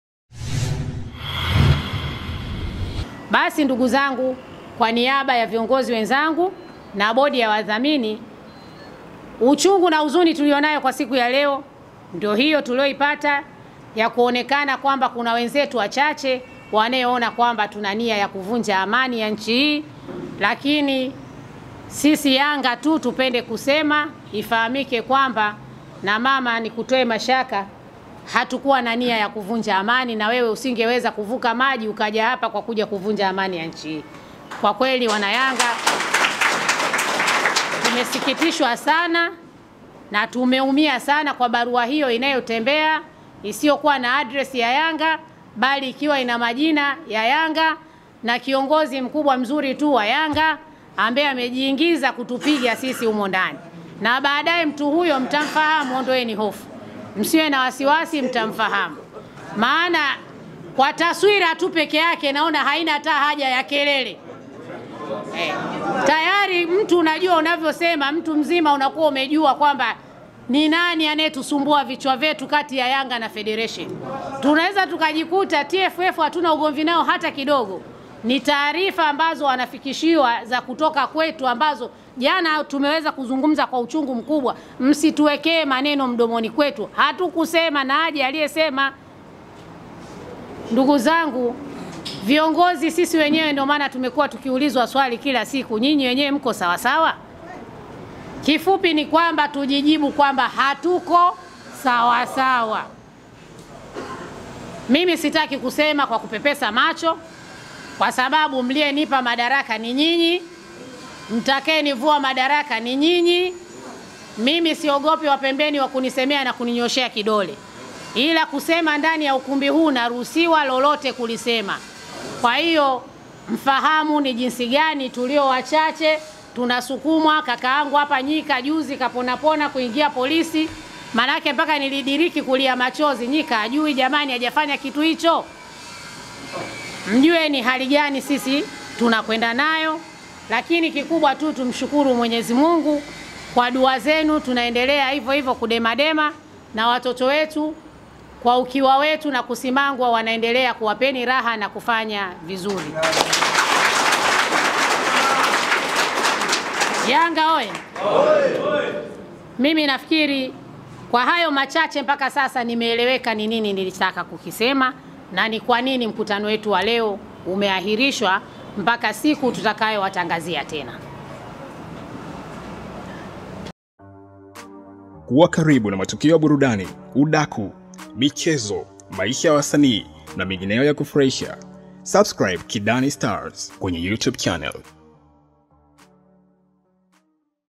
Sababu Mkutano Wa YANGA Kutofanyika Hizi Hapa
Mkutano huo ulitarajiwa kufanyika leo Jumamosi kwenye Ukumbi wa PTA, Sabasaba jijini Dar es Salaam kabla ya kusogezwa mbele.

Mkutano huo uliitishwa na Kamati ya Utendaji ya Yanga iliyokutana hivi karibuni na kukubaliana kufanya mkutano huo, kabla ya kwenda kwenye uchaguzi wa klabu hiyo uliopangwa ufanyike Januari 13, mwakani.